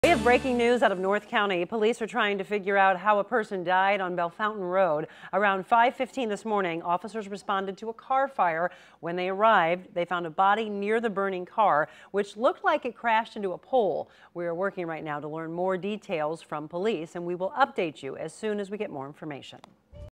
Police investigate apparent murder-suicide after crash on Bellefontaine
ST. LOUIS COUNTY, Mo. - Police are still trying to piece together what happened after they say a car crash on Bellefontaine Road Friday morning turned into the scene of an apparent murder-suicide.

The St. Louis County Police Department's North County Precinct responded at about 5:10 a.m. to the 11400 block of Bellefontaine Road for a report of a car on fire with a person trapped inside. Police arrived on scene and found a woman believed to be in her late 20s inside a car that had crashed into a utility pole. She had been shot and was pronounced dead at the scene.

A man believed to be about the same age was also in the car. He got out and ran when an officer tried to talk to him. He then shot himself in the head, police said. He was rushed to an area hospital, where he was pronounced dead.